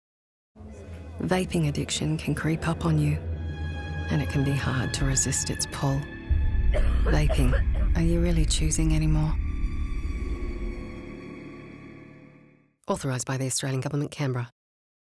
Vaping. Are you really choosing anymore? – 15 seconds
Vaping addiction can creep up on you. And before you know it, it can be hard to resist. Are you really choosing anymore?